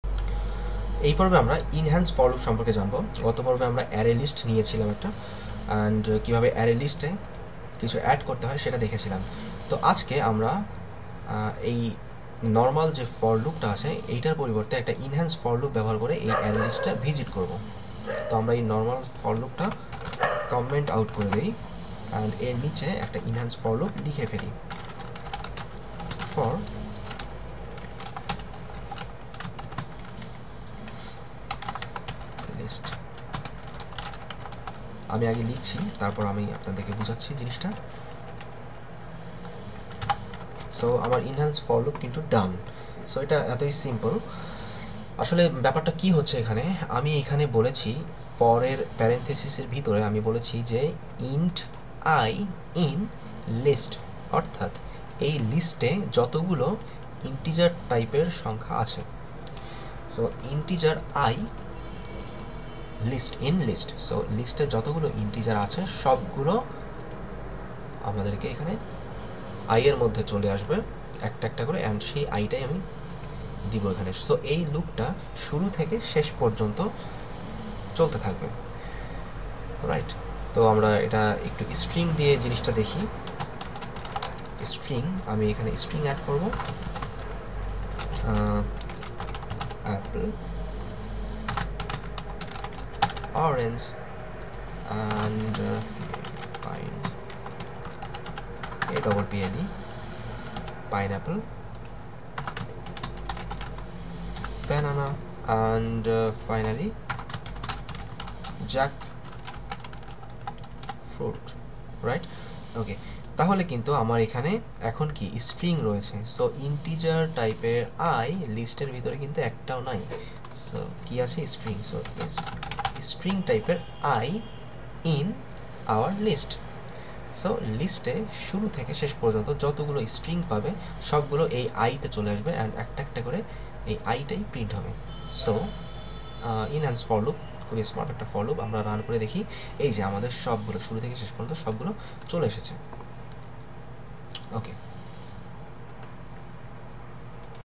5. part1.3, warm up , inhance for loop (javafx software development course in bangla)
Author : Nafiul Islam